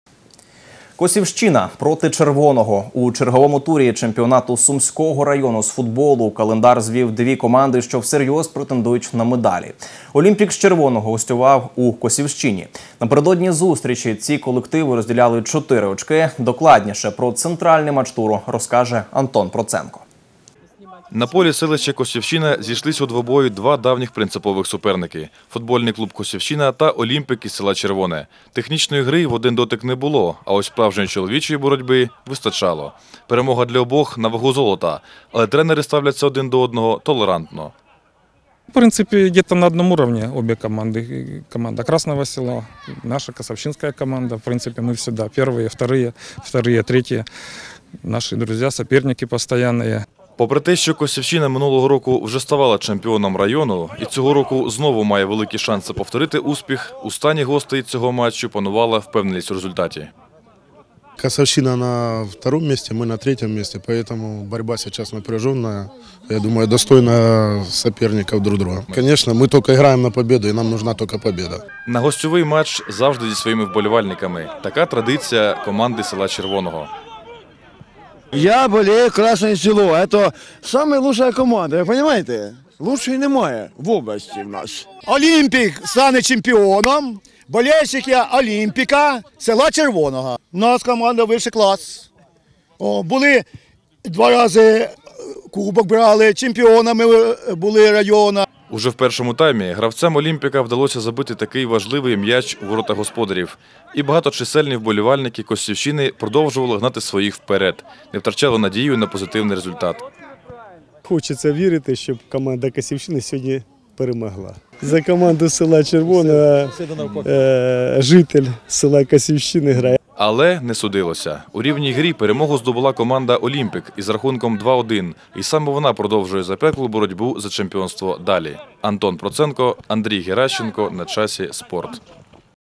У черговому турі чемпіонату Сумського району з футболу календар звів дві команди
«Олімпік» з Червоного гостював у Косівщині. Напередодні зустрічі ці колективи розділяли 4 очки.